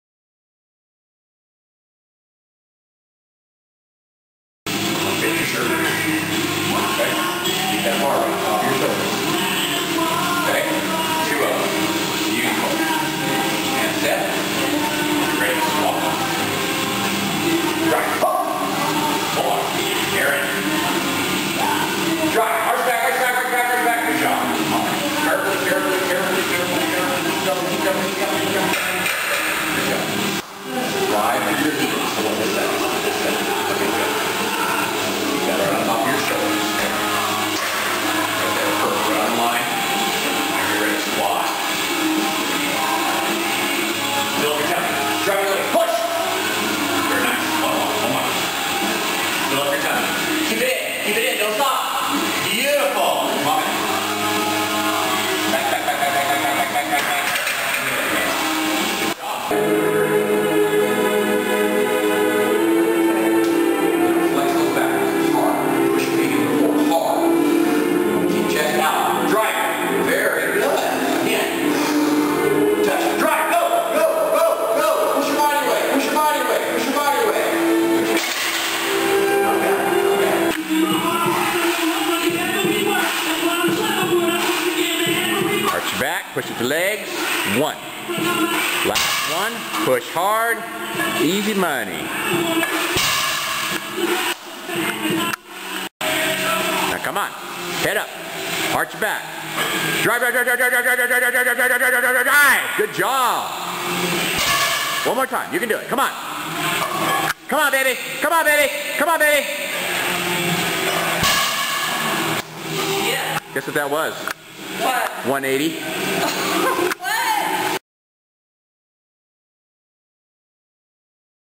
Js wout 5 30 09
My wife Jamies last workout before her first ever PL meet scheduled for June 7th.